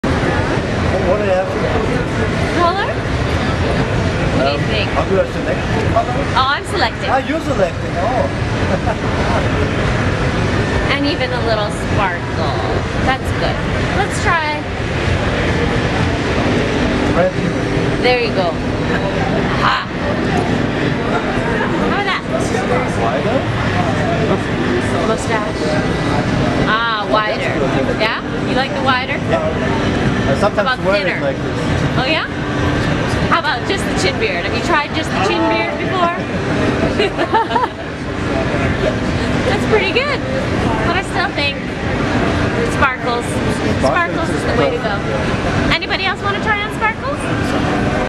Virtual Makeup Mirror at CES 2015
With this mirror you can try out new makeup without having to actually putting it on.